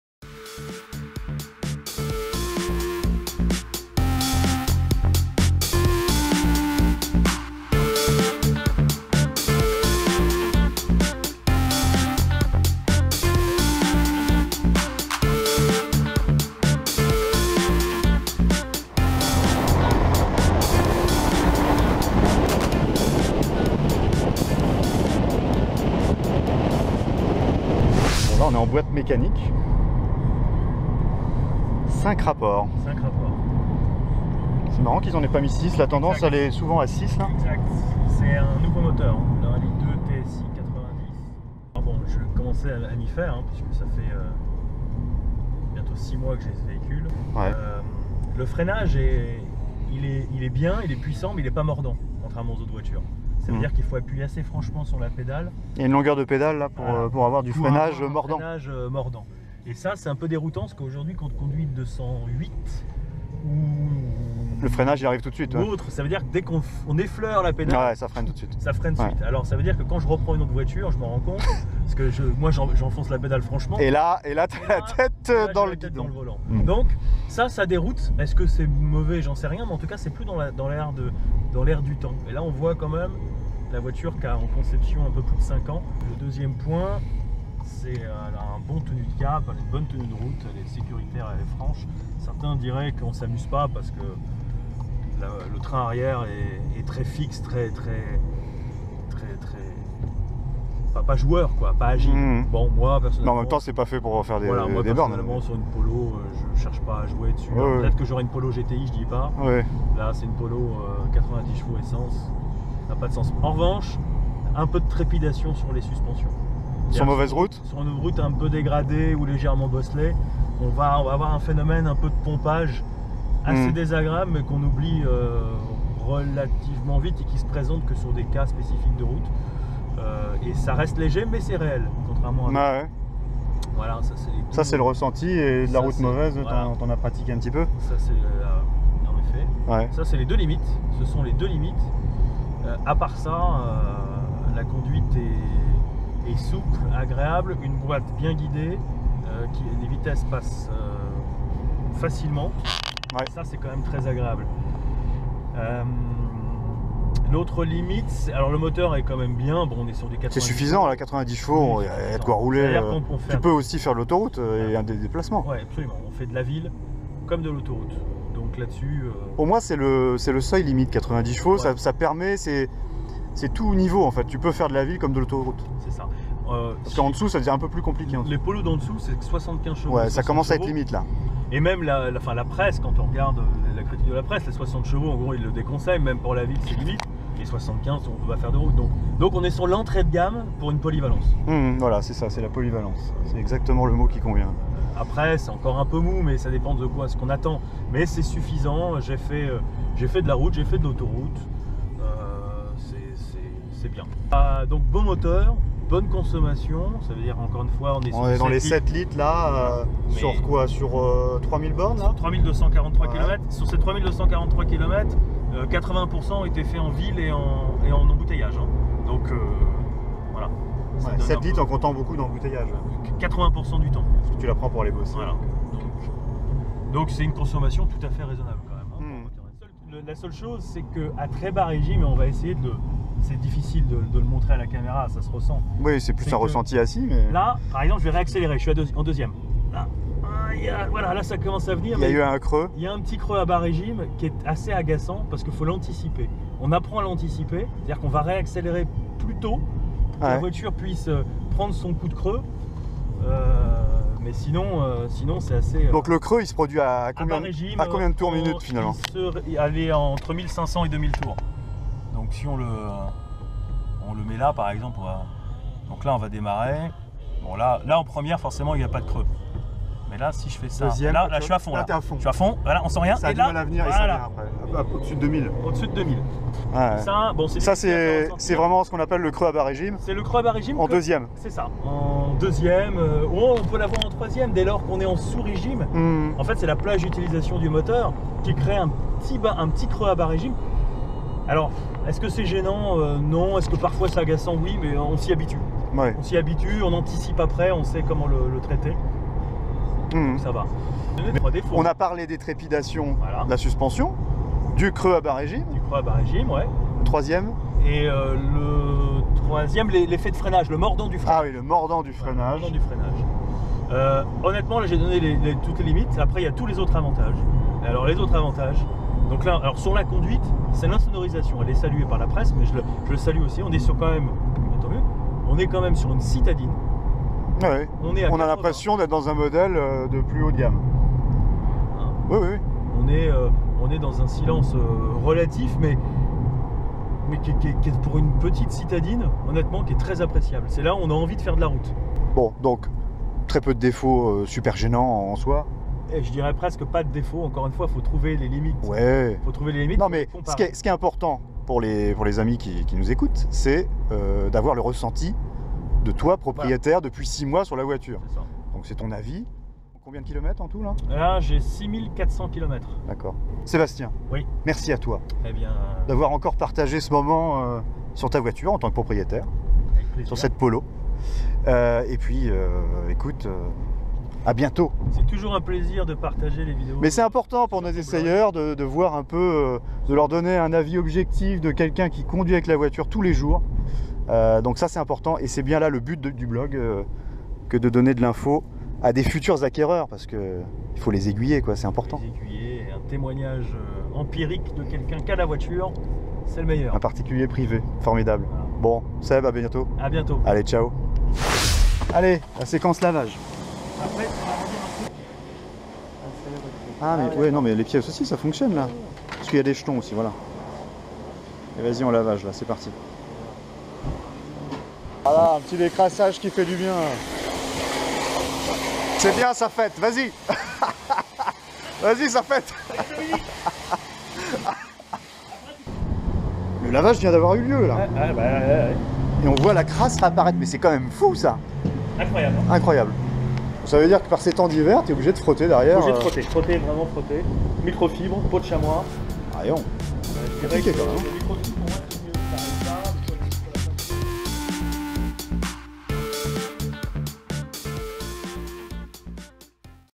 VW POLO 1.2 TSI 90 Ch ESSAI SUR ROUTE
l'essai sur route de la Polo 1.2 TSI 90ch Cup de Sébastien pour le blog